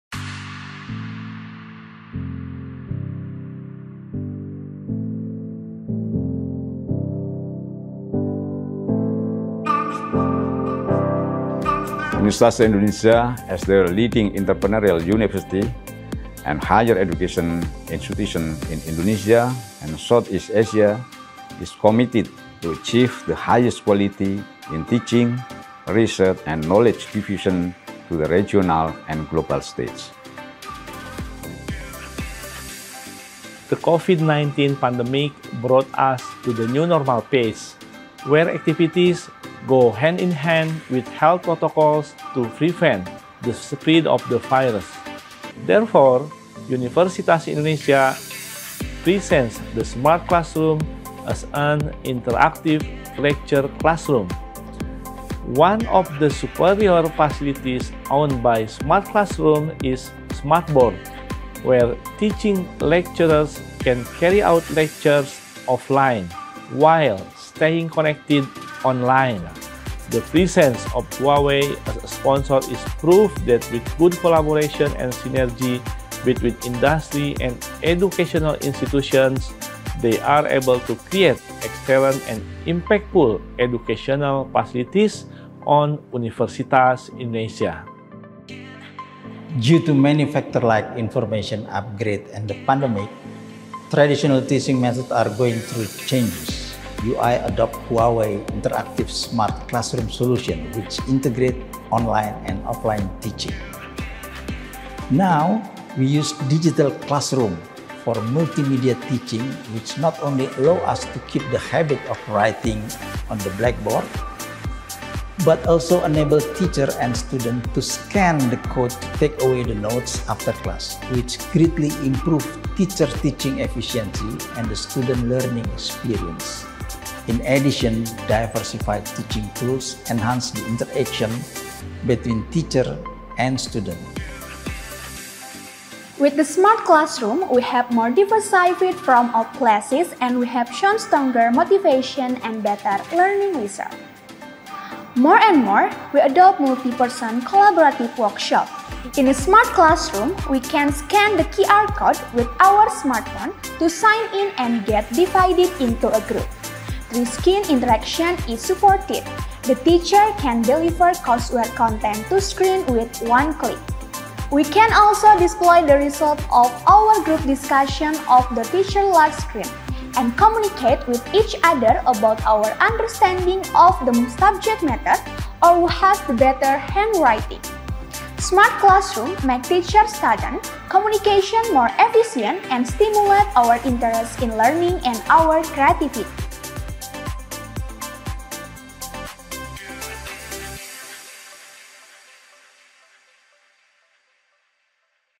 Huawei IdeaHub : UI Smart Classroom
Dalam proses pembelajaran, Universitas Indonesia (UI) telah menggunakan Huawei IdeaHub.
Huawei IdeaHub memberikan kemudahan dalam proses belajar mengajar. Mari simak pendapat dosen dan mahasiswa UI mengenai Huawei IdeaHub.